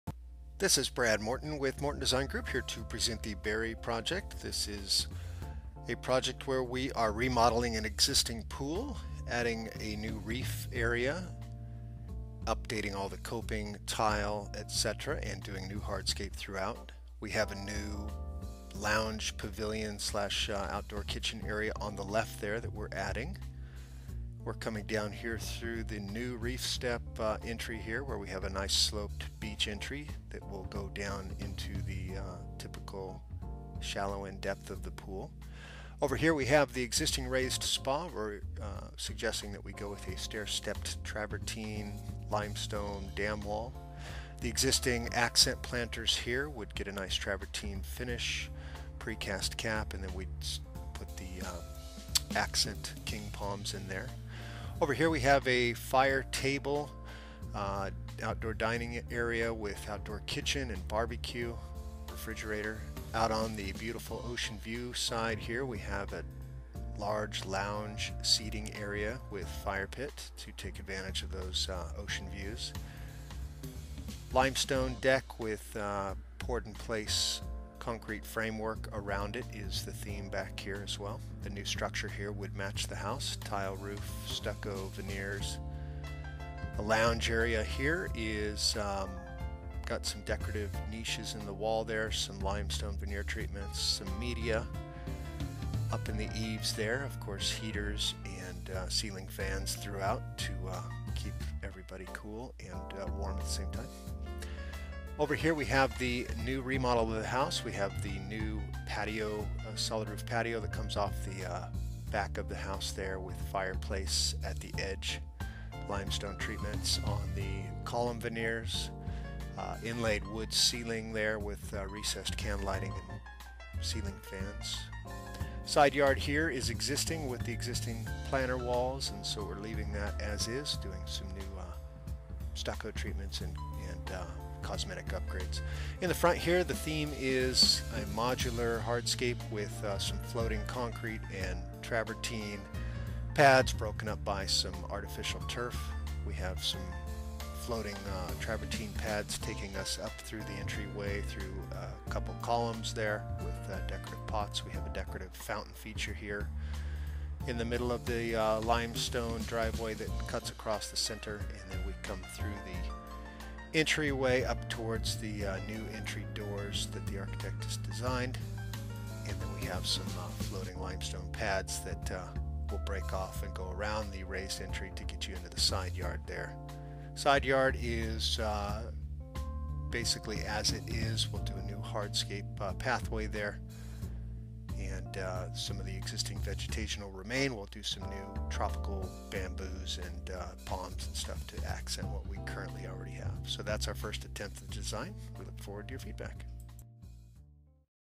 Berry Video 2 N
Morton Design Group presents our conceptual landscape design for the Berry Family.